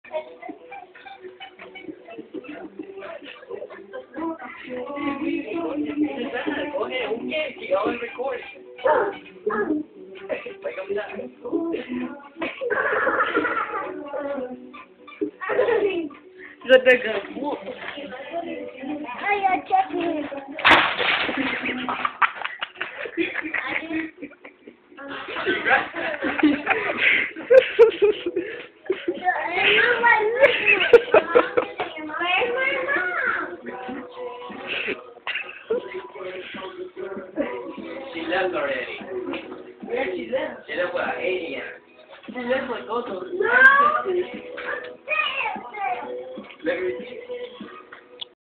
Teenie Clothesline Drop.3gp
Teenie AKA Brian Get's Clotheslined By Chris... Playing Around